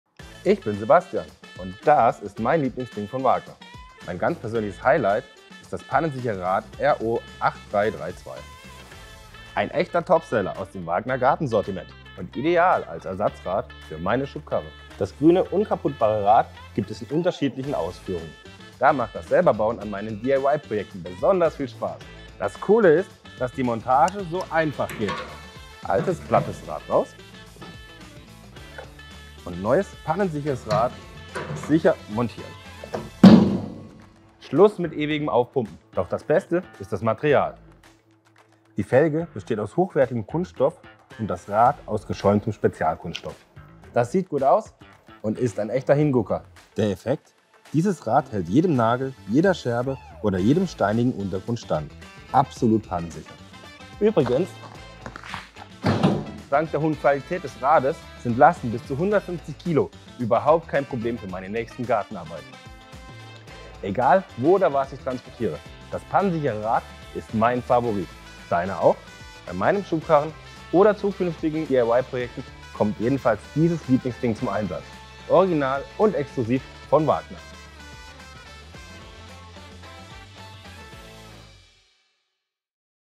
My favourite thing - the puncture proof wheel RO 8332
You can ask me about every single WAGNER product – I know them all. But if you ask me for my personal highlight: My favourite thing is the puncture-proof wheel "RO 8332"!

Why? Quite simply because it is absolutely "indestructible", has maximum all-terrain mobility, is extremely strong, and also never needs to be inflated! I'll show you right here in the video.